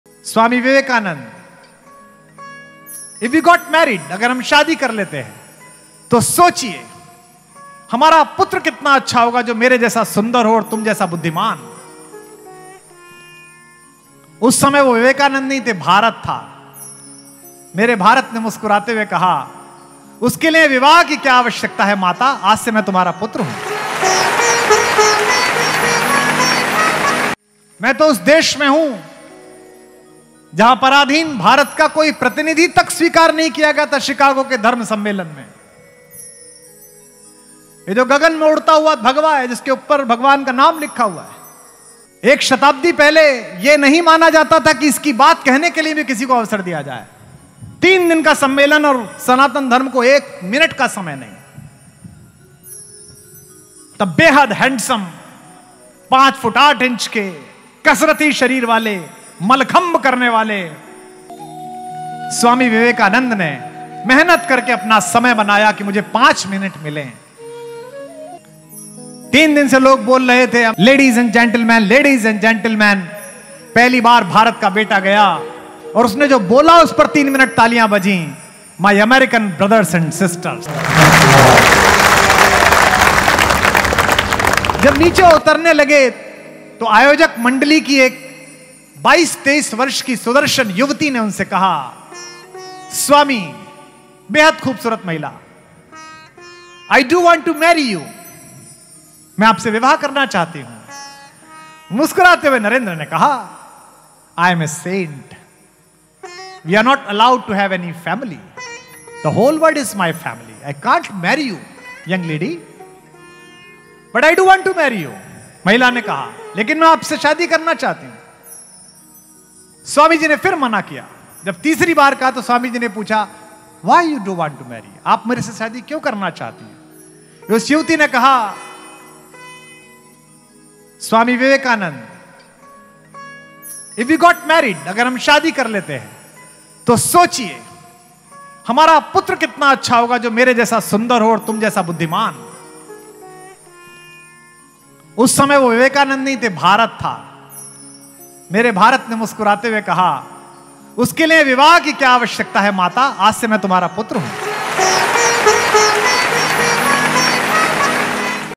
जब विवेकानंद को मिला था शादी का प्रस्ताव | Dr Kumar Vishwas | Best Of KV
हिन्दुस्तान की सरहदों से हज़ारों किलोमीटर दूर अमेरिका के विश्व धर्म सम्मेलन के उपरांत स्वामी विवेकानंद का यह प्रसंग भारत की उस महान संत परंपरा के वजूद के प्रति एक असंदिग्ध आश्वस्ति है जहॉं साधना में रत अल्हड़-निर्विकार योगियों के क्रुद्ध दृष्टिपात से कामदेव तक पराजित होकर भस्म हो जाते हैं।